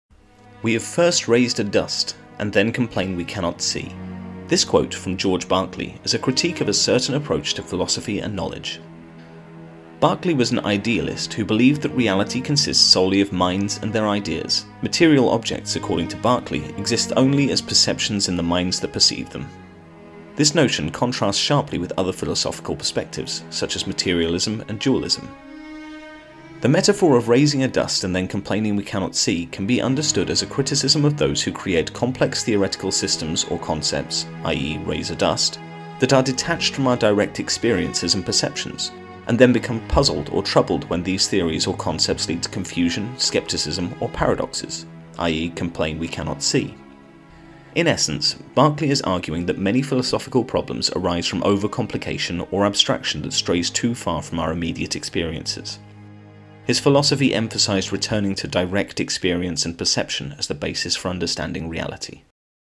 George Berkeley “We have first raised a dust and then complain we cannot see." meaning
This quote from George Berkeley is a critique of a certain approach to philosophy and knowledge. Berkeley was an idealist who believed that reality consists solely of minds and their ideas; material objects, according to Berkeley, exist only as perceptions in the minds that perceive them. This notion contrasts sharply with other philosophical perspectives, such as materialism and dualism.

The metaphor of "raising a dust and then complaining we cannot see" can be understood as a criticism of those who create complex theoretical systems or concepts (i.e., "raise a dust") that are detached from our direct experiences and perceptions, and then become puzzled or troubled when these theories or concepts lead to confusion, scepticism, or paradoxes (i.e., "complain we cannot see").

In essence, Berkeley is arguing that many philosophical problems arise from overcomplication or abstraction that strays too far from our immediate experiences. His philosophy emphasized returning to direct experience and perception as the basis for understanding reality.